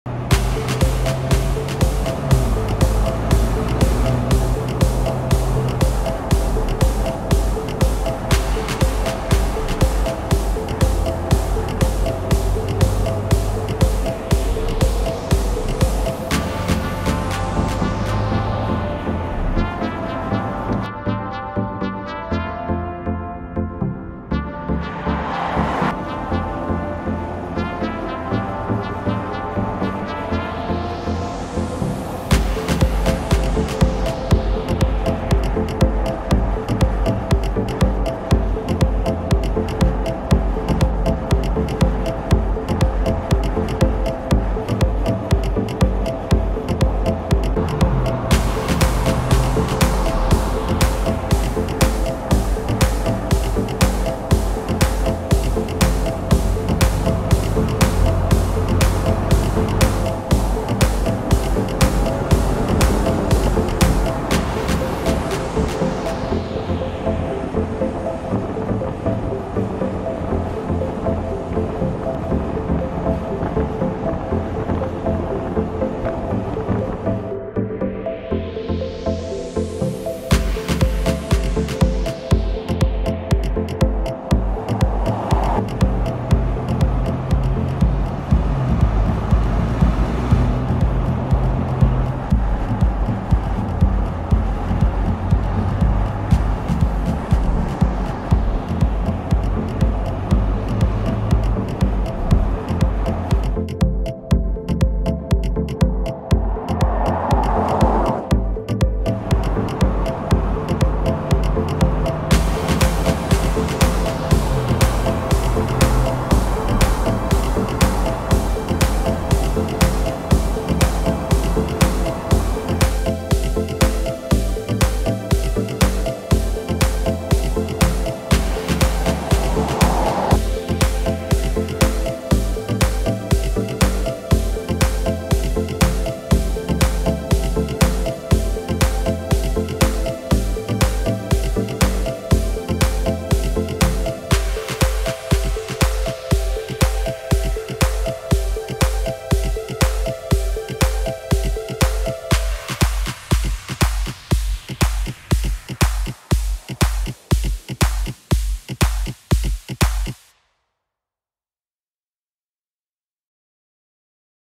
2024 Audi Q8: Sleek Style Meets Electrifying Performance
The 2024 Audi Q8 is a luxury SUV that is known for its sleek design, powerful engine, and spacious interior. It is available in two trim levels: Premium and Prestige.

The Q8 is powered by a 3.0L turbocharged V6 engine that produces 335 horsepower and 369 lb-ft of torque. It is paired with an 8-speed automatic transmission and Quattro all-wheel drive. The Q8 can accelerate from 0 to 60 mph in 5.7 seconds.

Source: AUDI
To stay up to date with a lot of new videos Do you like it? Comment, like, dislike, and share! 🤟